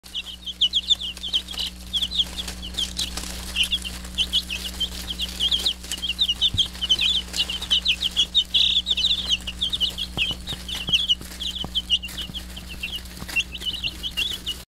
Bringing the pasture to week old chicks
Rather than putting chicks out on pasture at a tender age and losing them to predators, we bring the pasture to them.  At this age, their favorite foods are sourgrass and tick trefoil flowers, buds, and seeds.